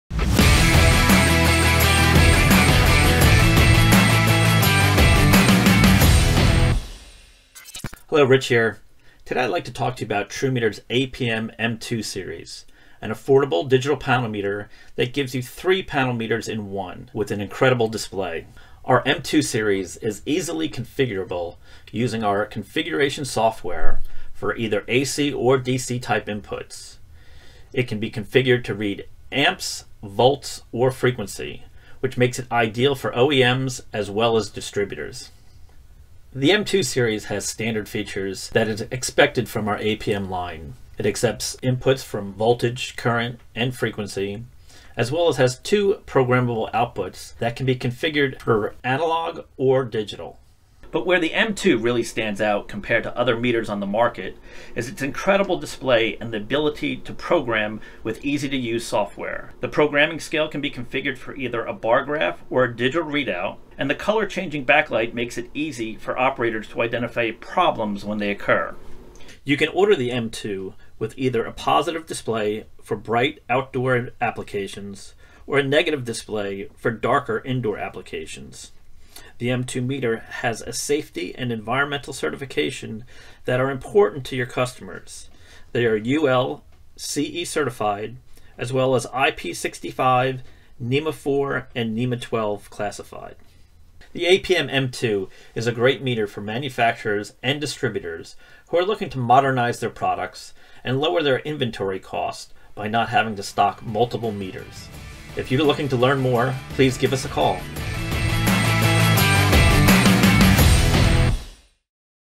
Trumeter APM M2 Meter
The APM M2 Meter is an all-in-one meter that can be configured to measure amps, voltage or frequency for applications including switchgear and power control centers. It is 100-240V AC mains powered with no additional power supply required.

Every member of the APM family has unique dynamic backlighting and starburst messaging to instantly alert operators to problem conditions. And the alerts, scale and range are fully customizable through the plug and play software.

The M2 Meter comes with a positive display for bright outdoor environments or a negative display for higher contrast indoors.